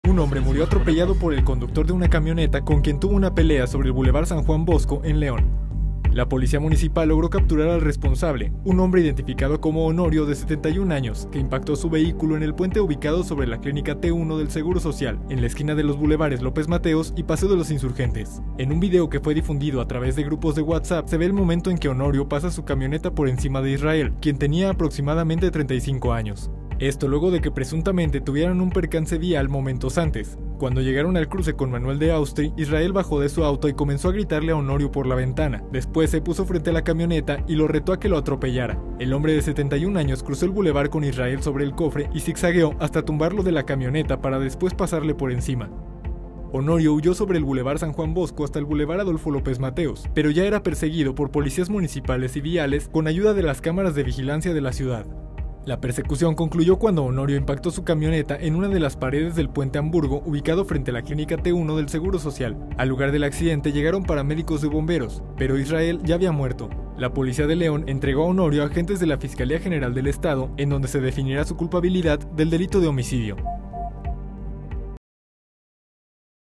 Conductor de camioneta atropella y mata a hombre tras conflicto vial en León
Israel murió atropellado por Honorio cuando el primero bajó de su auto para retarlo a que lo atropellara tras un conflicto vial y murió cuando el conductor le pasó las llantas por encima.